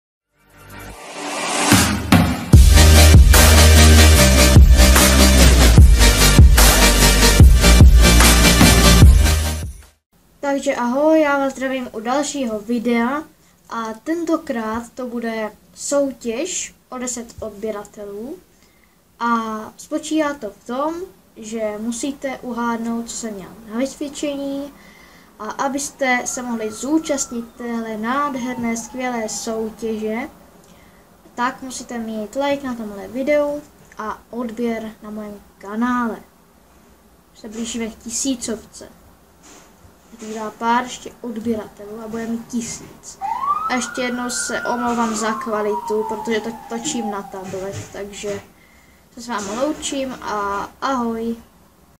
Soutěž o 10 odběratelů!!!
takže pokuste se uhodnout co jsem měl na vysvědčení